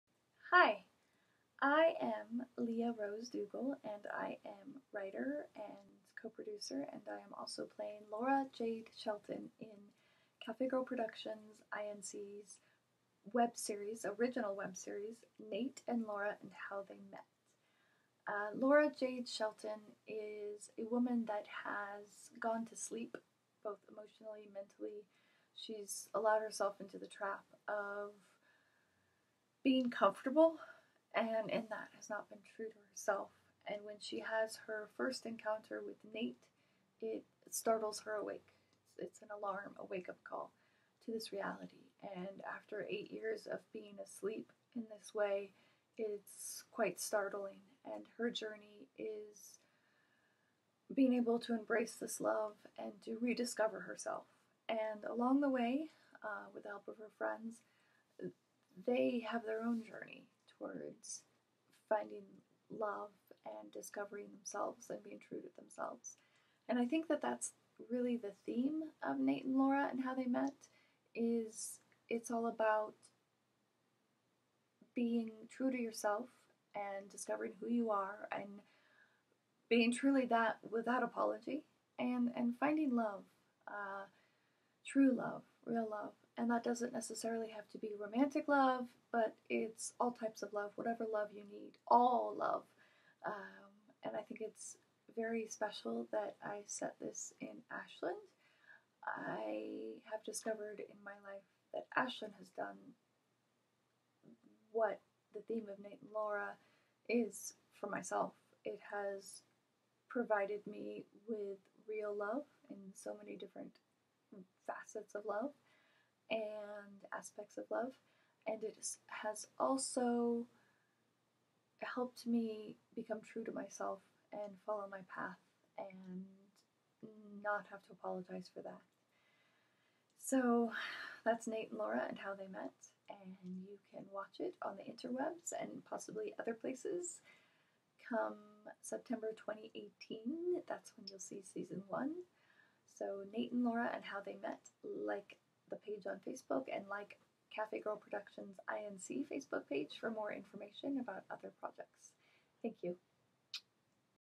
Interview With Actress, Lia Rose Dugal
Interview With Actress, Lia Rose Dugal, about her character, Laura Jade Shelton and the theme of Nate & Laura & How They Met